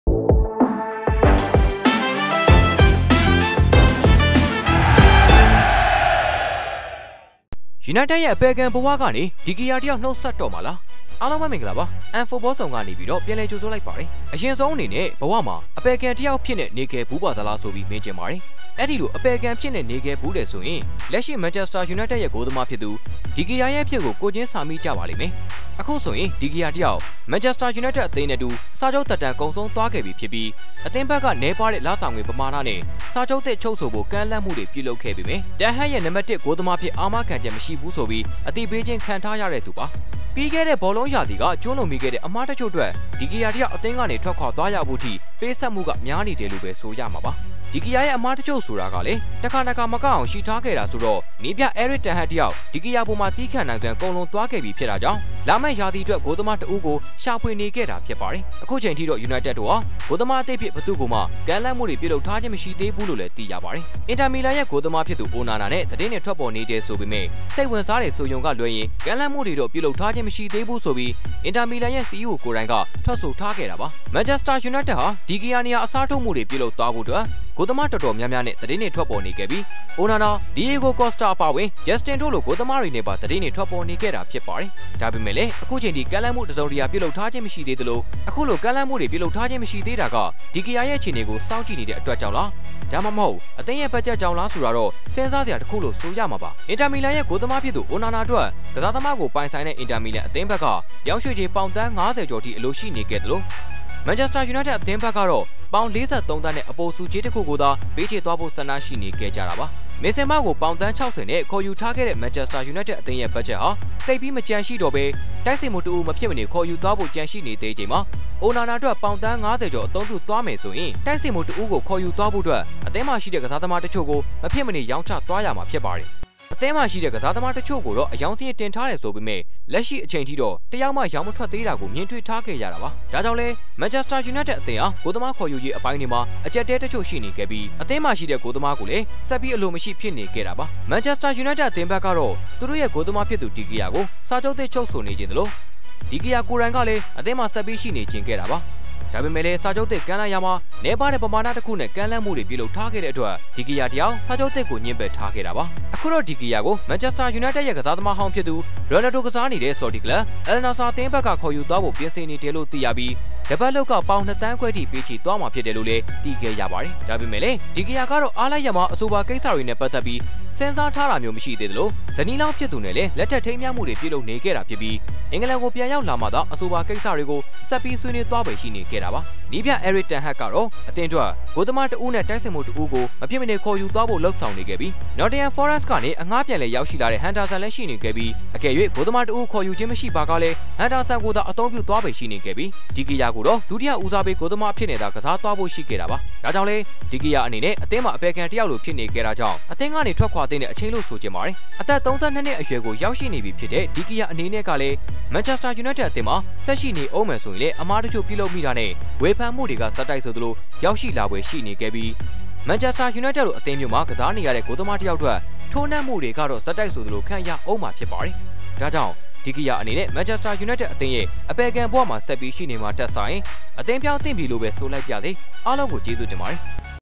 ယူနိုက်တက်ရဲ့ အပယ်ခံဘဝကနေ ဒီဂီယာတစ်ယောက် နှုတ်ဆက်တော့မှာလား ?
20th of June: Interesting Manchester United news.